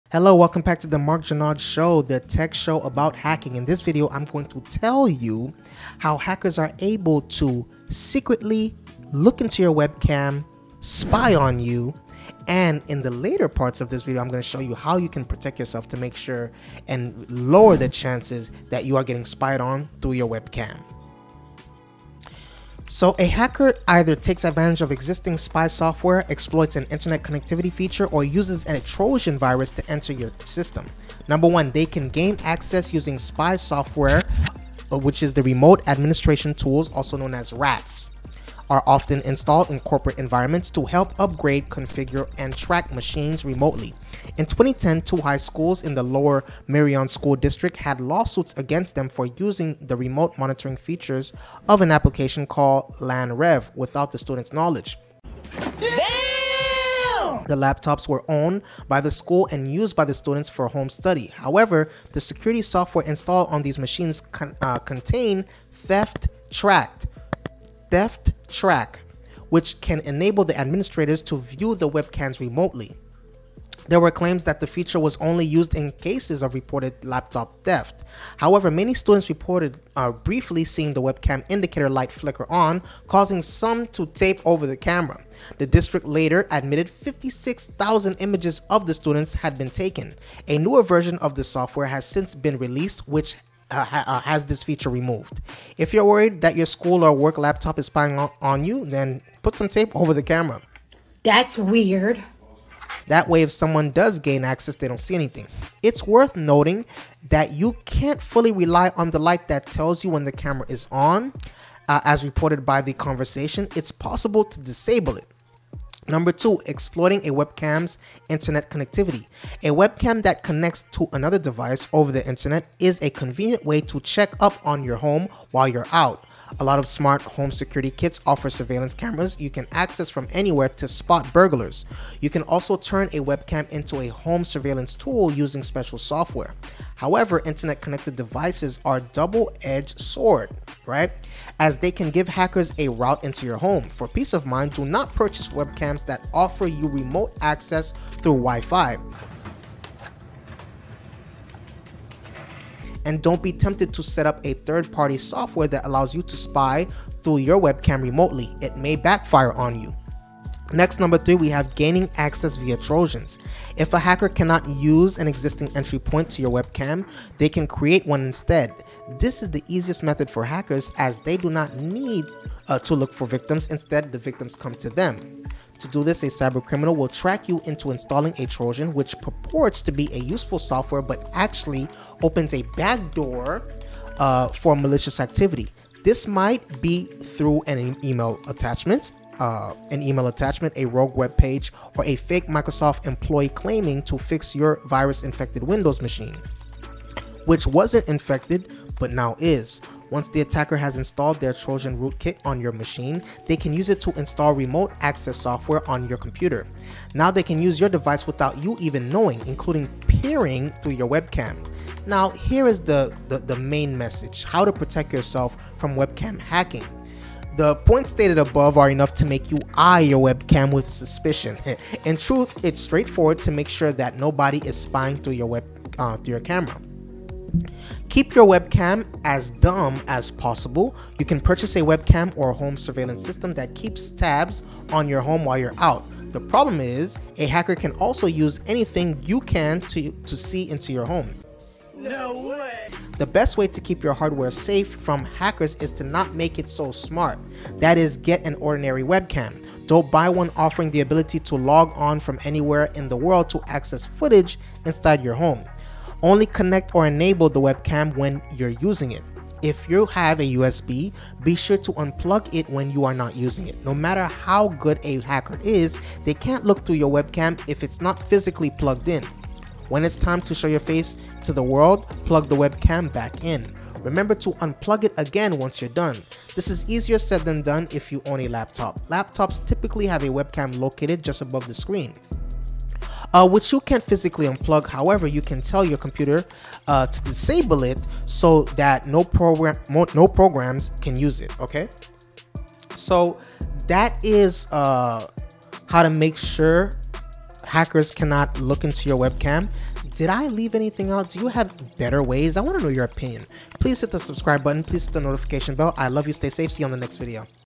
Hackers Turn On Your Webcam! Scammers 101
Hello Hack Pack! What I will cover in my latest video are the little known ways of how hackers are able to gain access to your webcam and turn it on without you knowing. Please enjoy my latest video where I also go over how to protect yourself from being secretly watched!